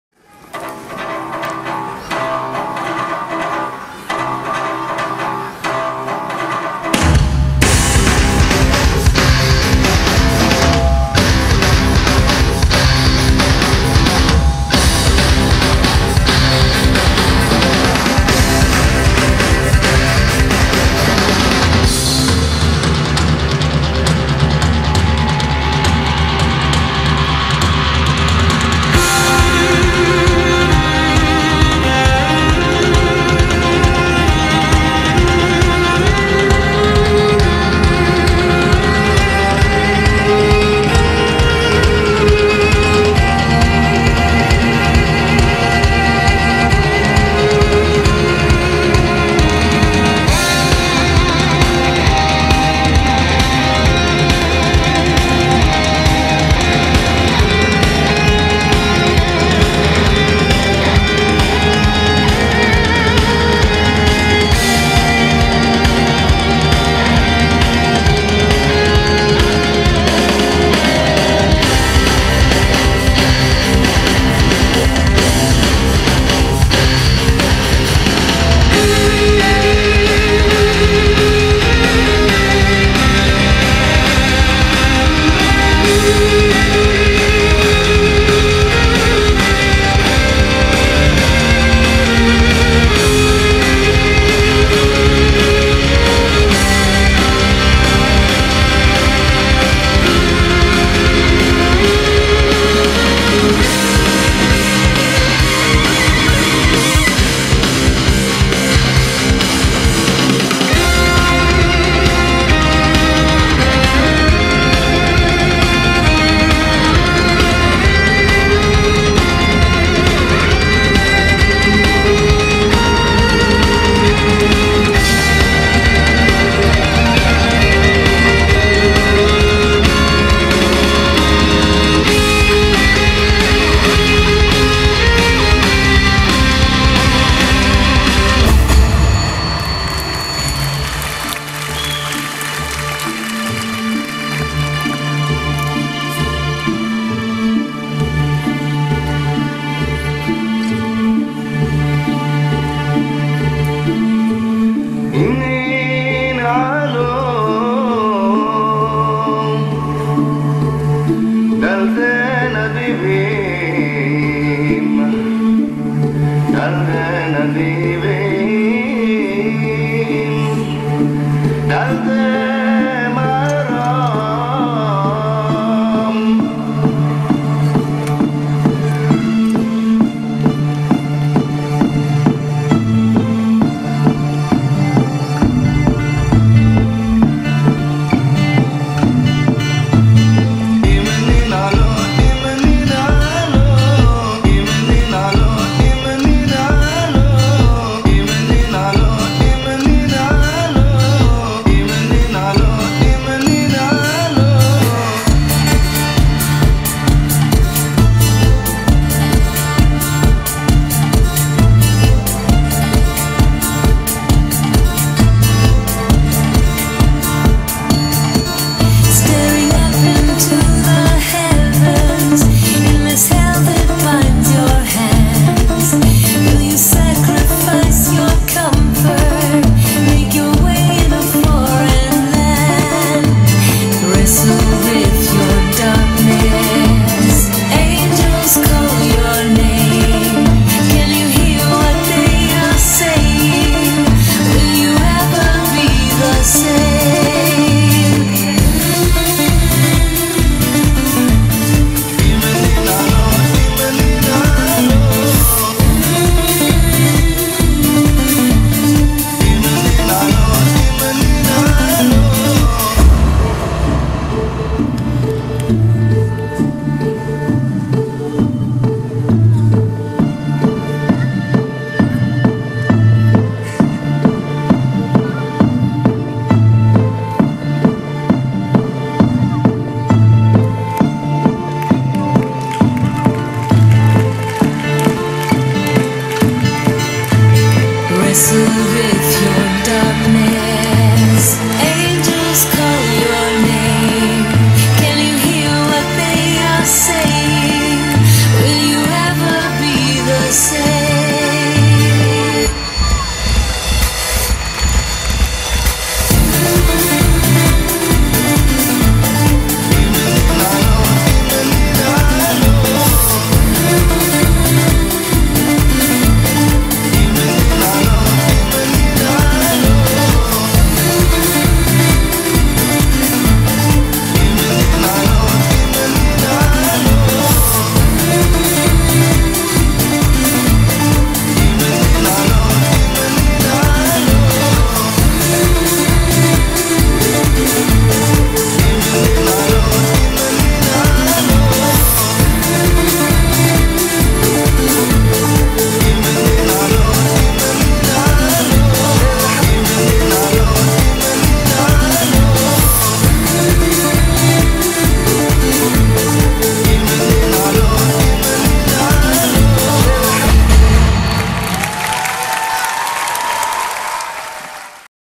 Original Hula Hoopt act by Ekaterina Sheludyakova
New original Hulat Hoop act with handstand and Ariel trick.
directed by Tatyana Kuznetsova.